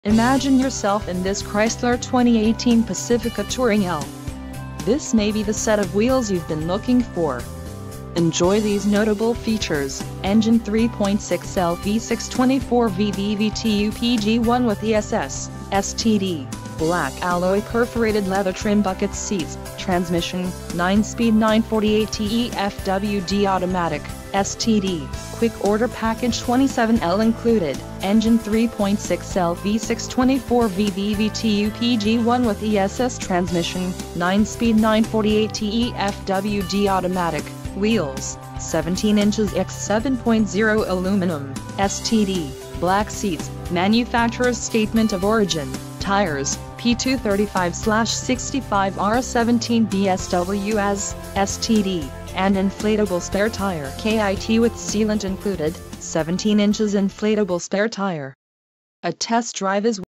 2018 Chrysler Pacifica Touring L
CARFAX One-Owner. Clean CARFAX. Granite Crystal Metallic Clearcoat 2018 Chrysler Pacifica Touring L FWD 9-Speed Automatic 3.6L V6 24V VVT One Owner !